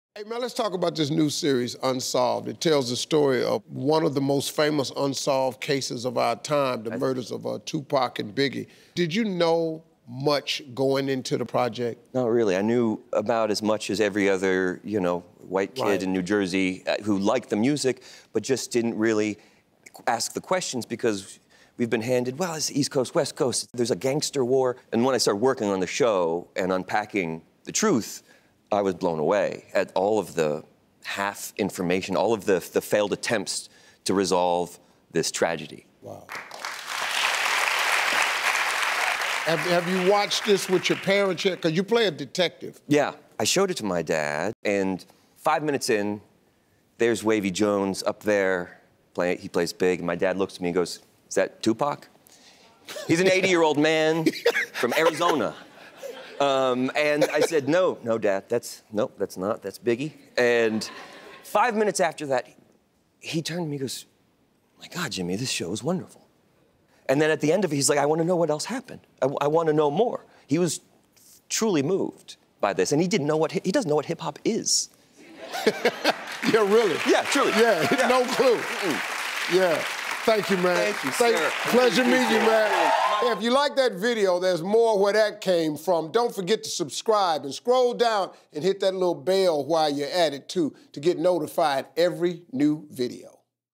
Jimmi Simpson Dishes On His Show, 'Unsolved: The Murders of Tupac and the Notorious B.I.G.'
Unsolved: The Murders of Tupac and the Notorious B.I.G.' sounds like one crazy show, the way Jimmi Simpson tells it!